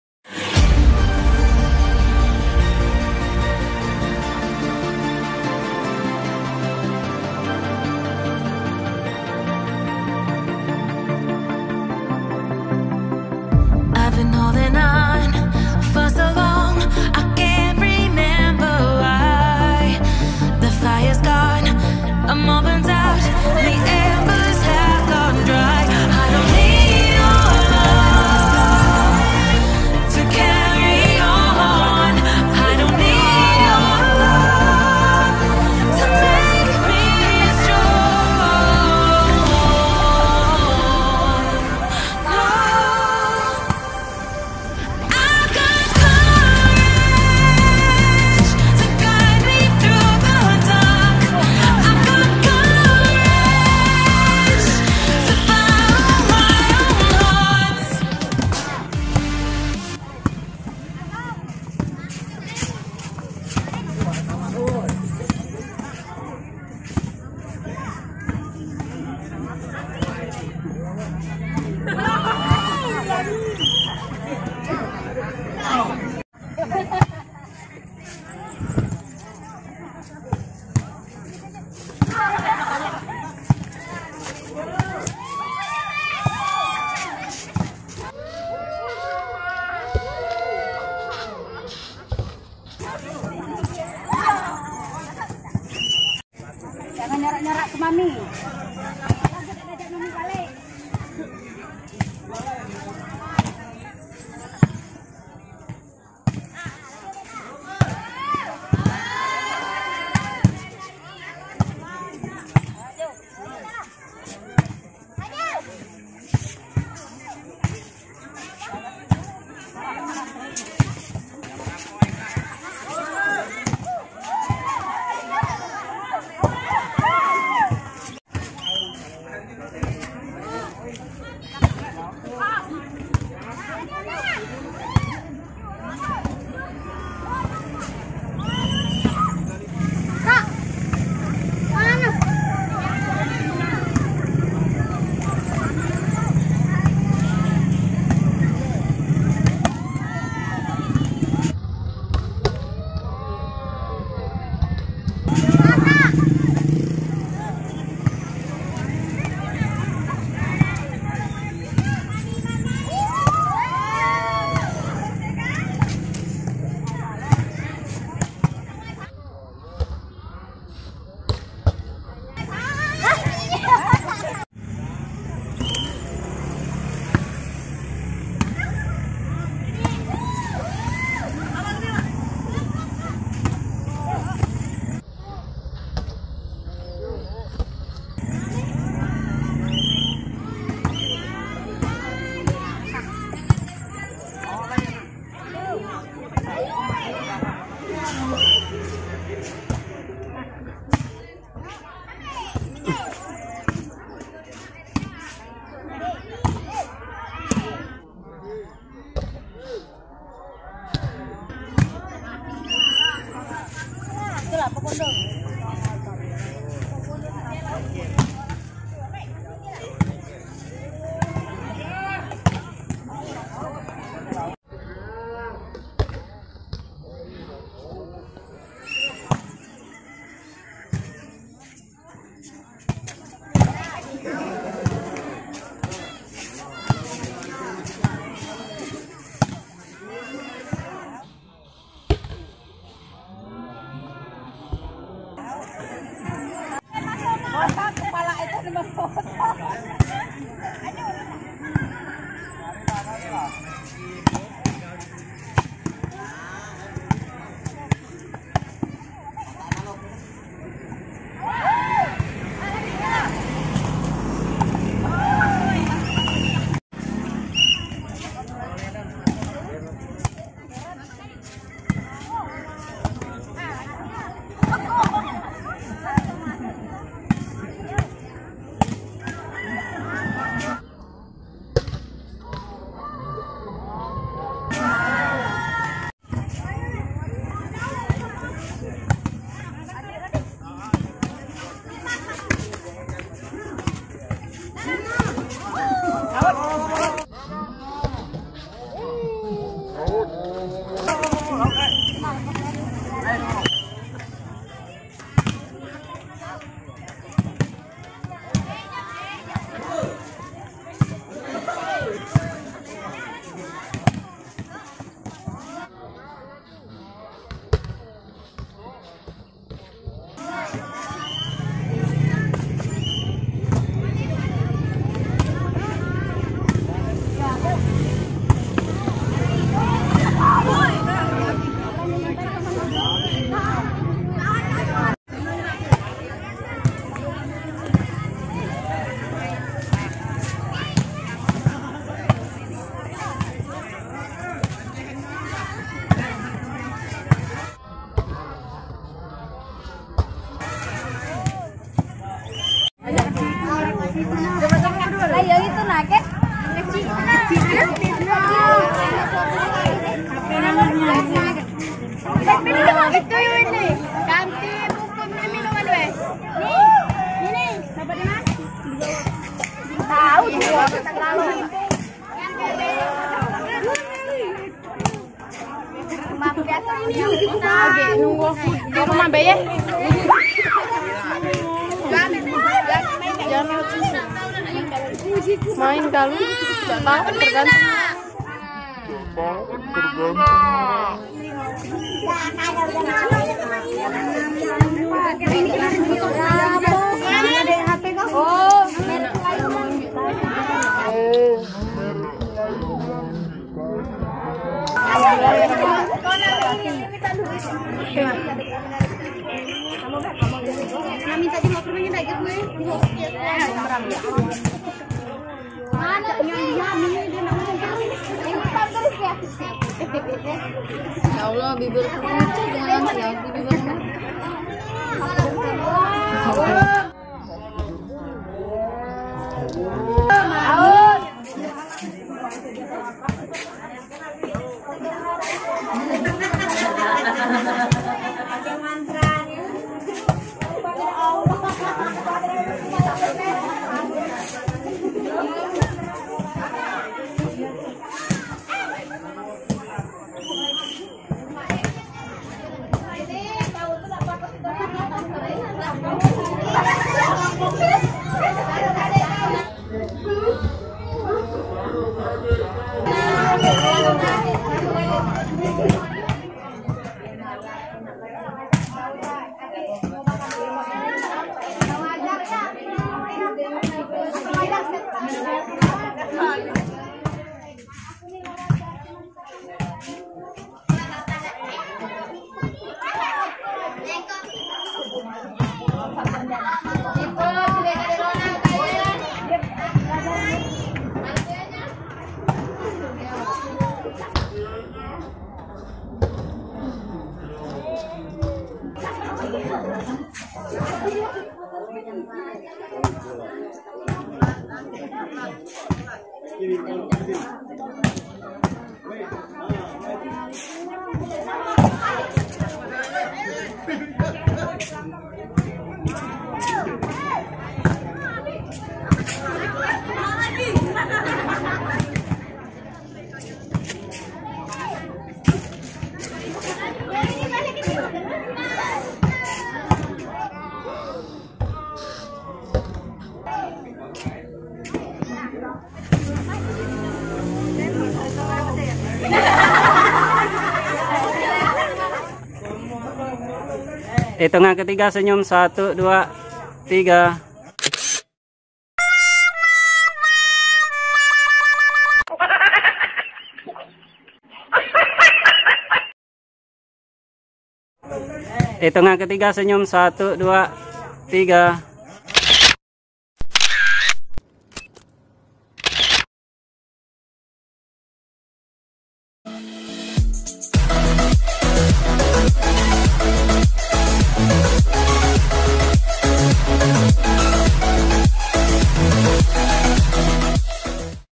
Sparing Momen Volley Ball 2020 Letang ( Talang Jawo ) vs Supat Timur, Kab Musi Banyuasin...
pada kesempatan kali ini kmi berkesempatan  save moment team letang talang jawo vs supat timur 20 nov 2020... mau keseruannya yuk tonton video kami sampe  selesai...